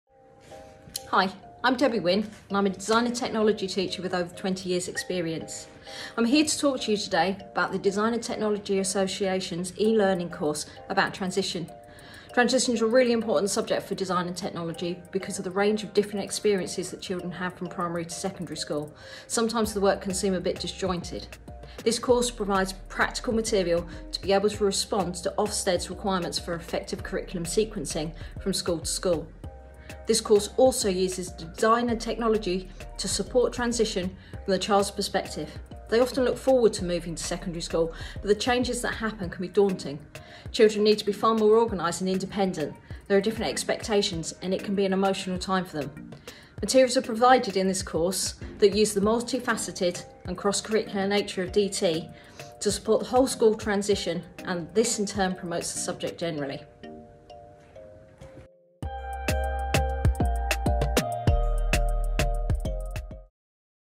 Transition in Design and Technology
This course will help you smooth the transition from Primary to Secondary school.